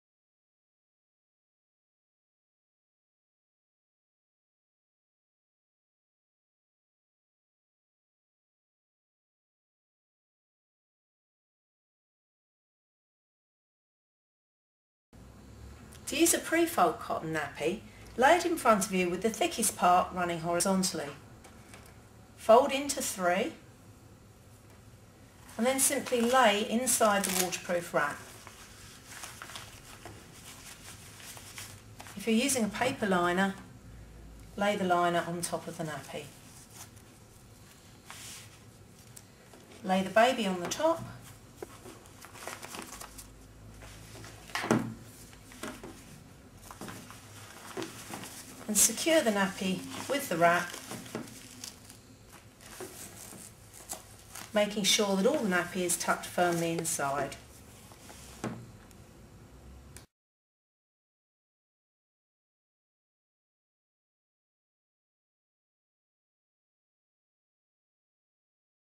How to use a pre fold real nappy
Kay Beighton, Essex County Council Cloth Nappy Adviser